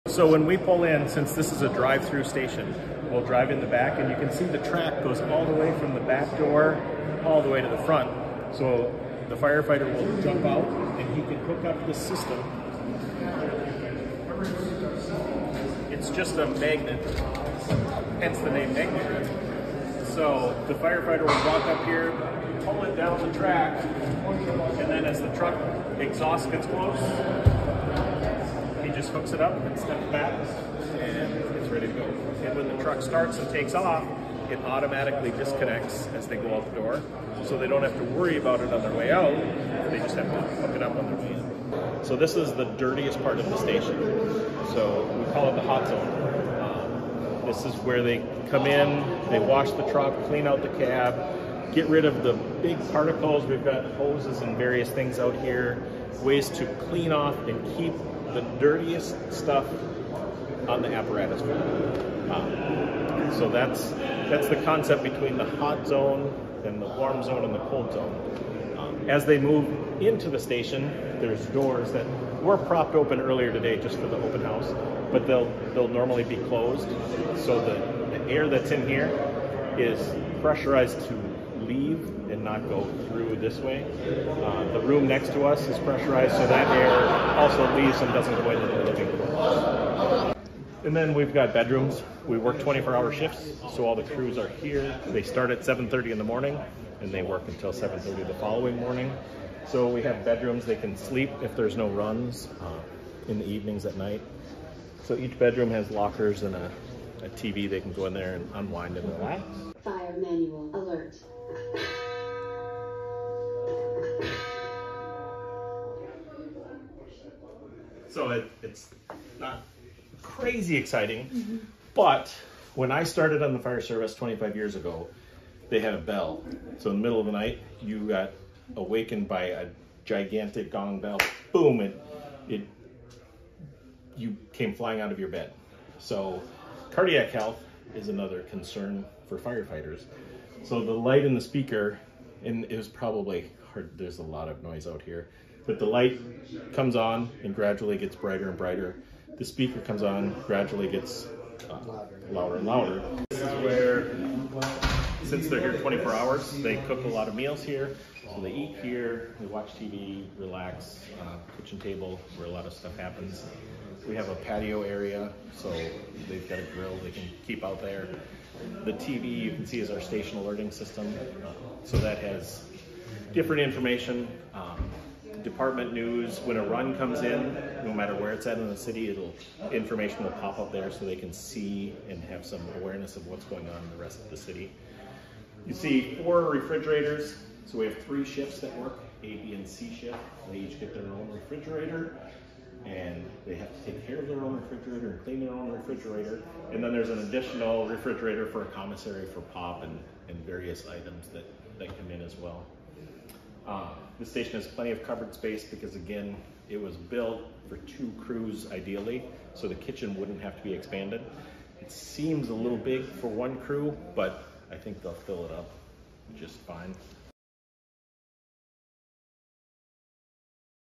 Fargo opens new $5.4M fire station at nearly 7% under budget
With a snip of yellow and black caution tape bearing the words “Fire Line Do Not Cross,” Fargo city officials celebrated a brand new fire station this week.

The grand opening for Fargo Fire Department Fire Station 8 at 6617 33rd St. S., was held Wednesday, Oct. 23, with an open house for the public.